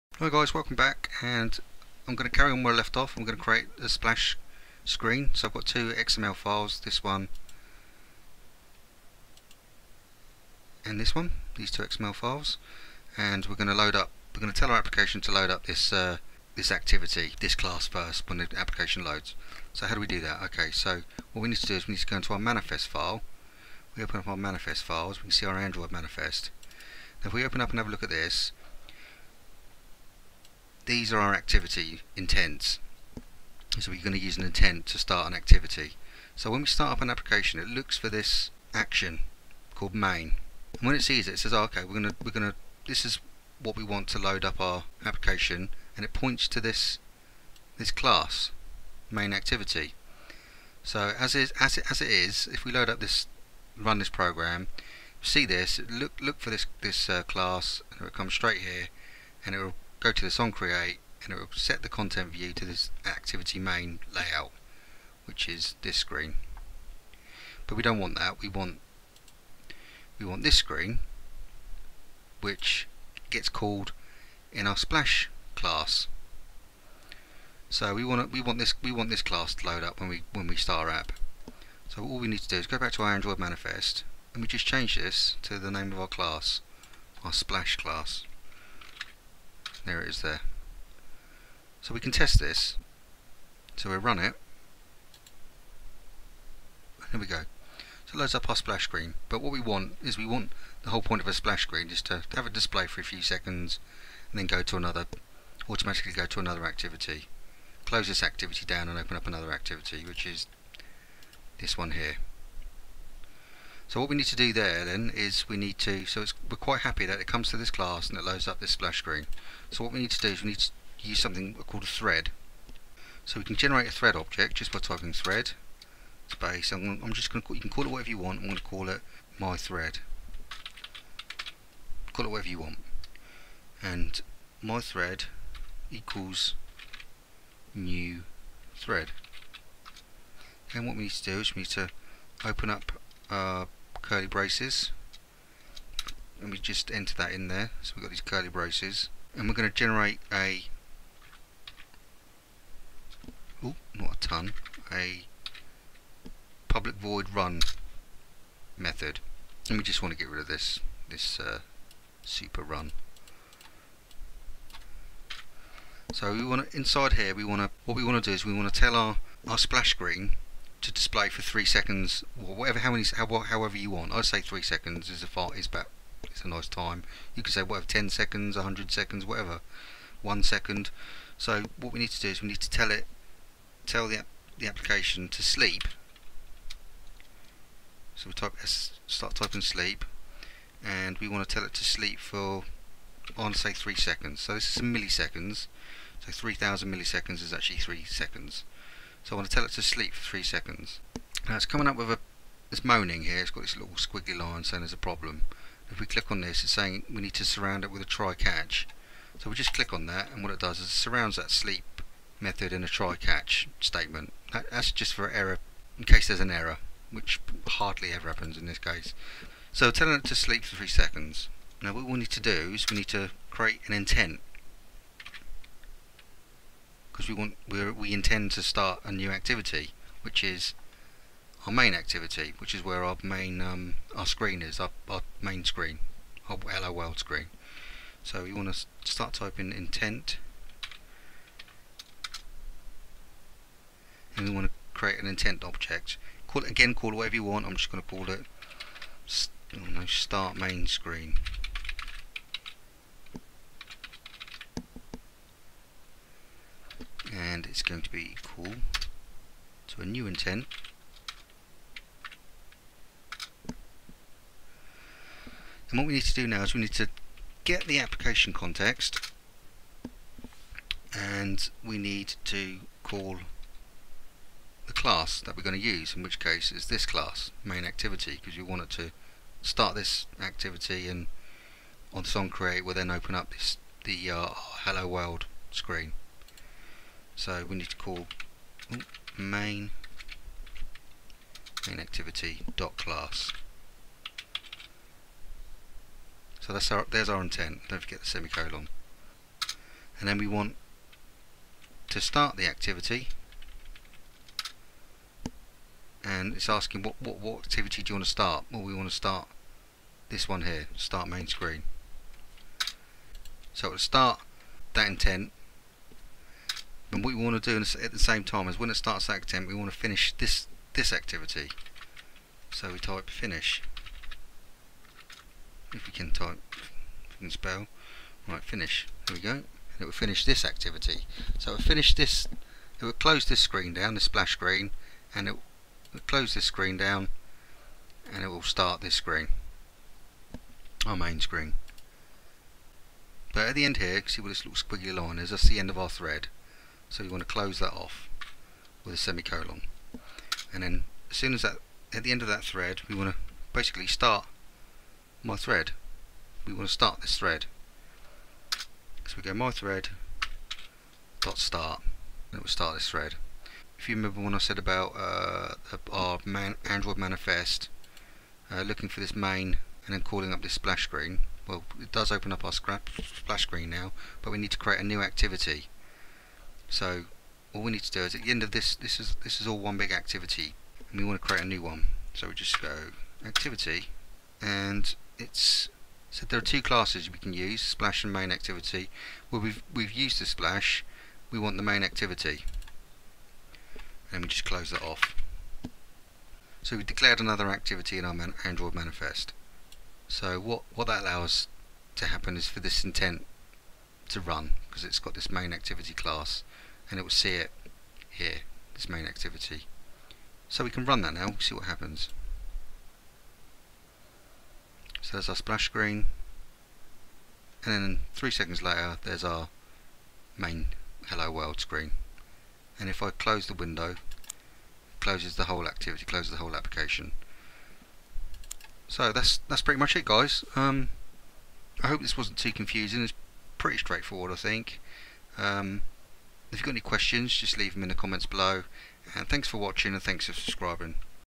Learn Android Studio: (25) How to create a splash screen - Part 2
In this video we'll continue to look at creating a splash screen for your android app and delve into the use of Threads and Intents.